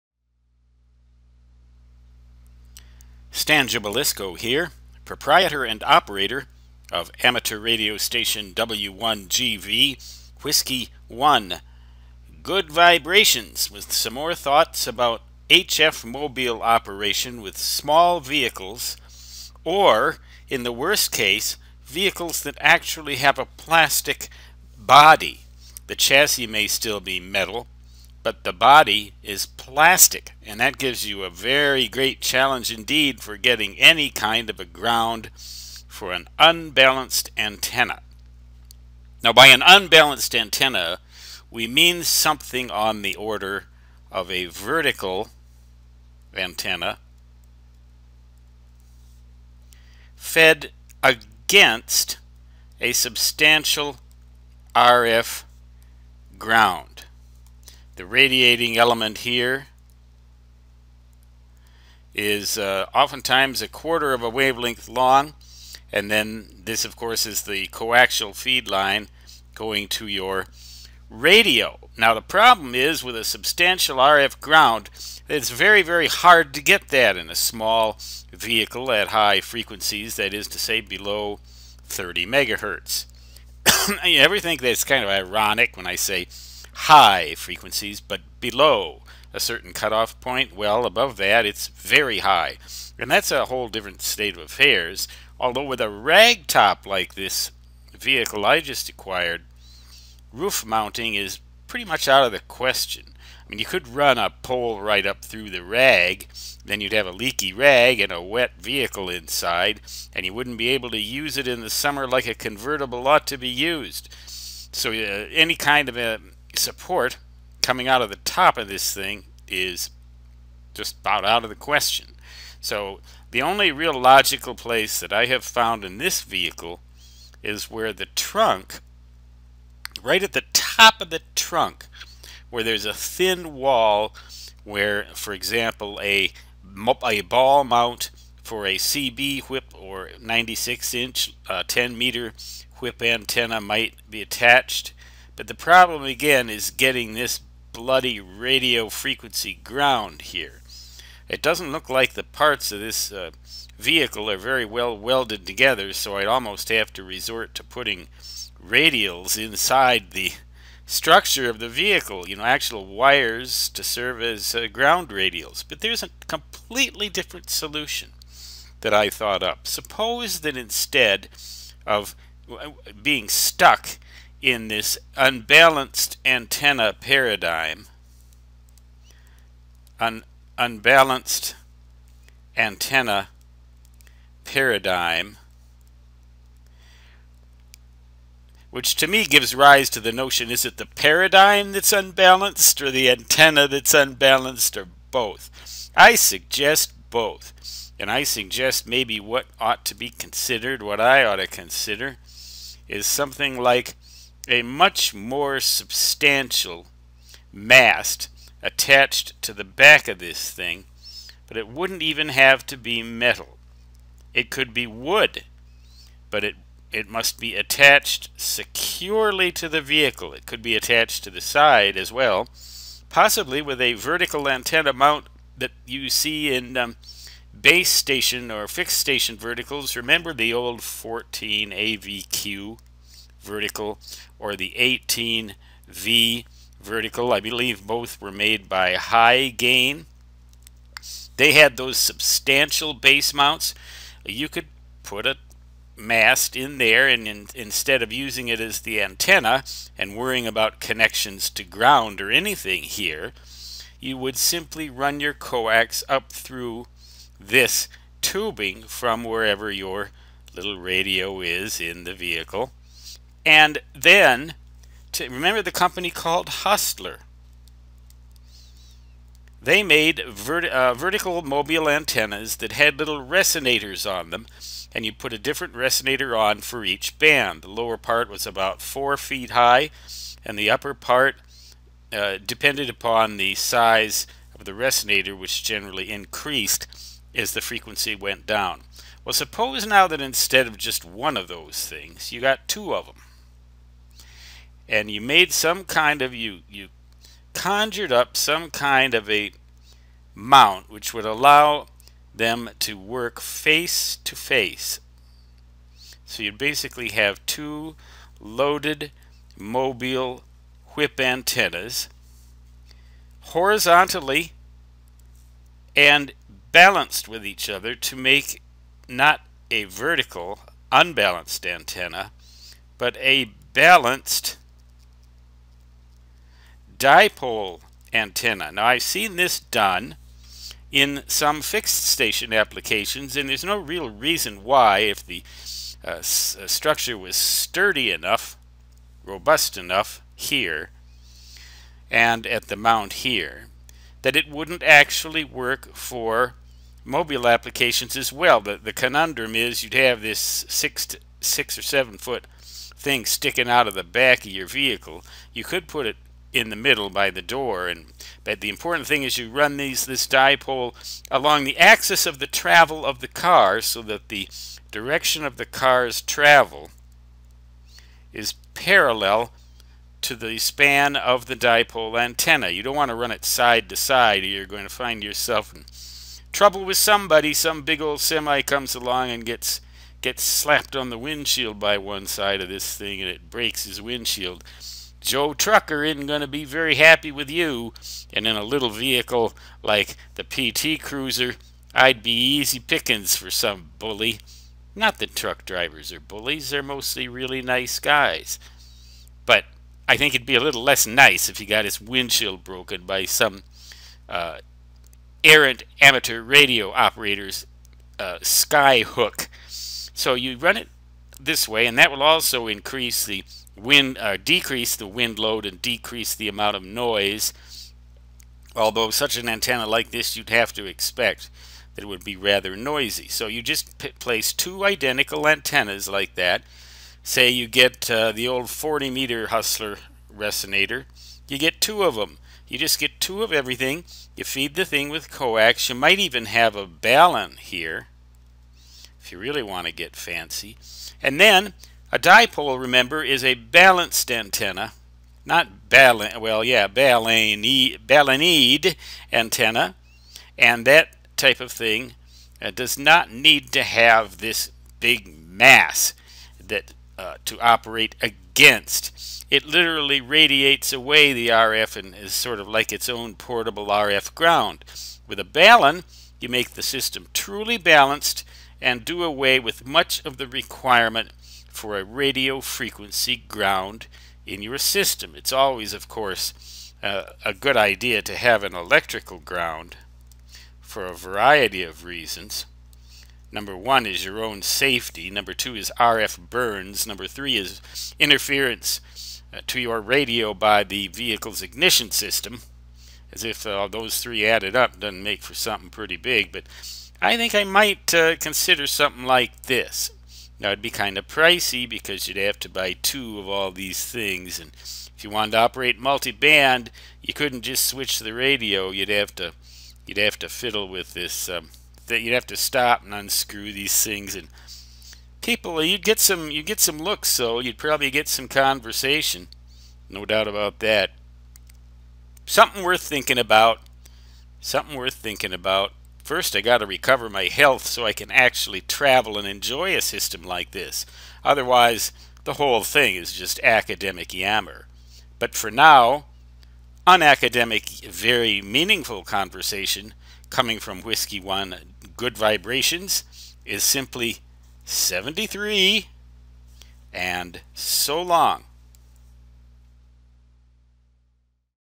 Dipoles for HF Mobile
Two 1/4-wave whips make a 1/2-wave dipole.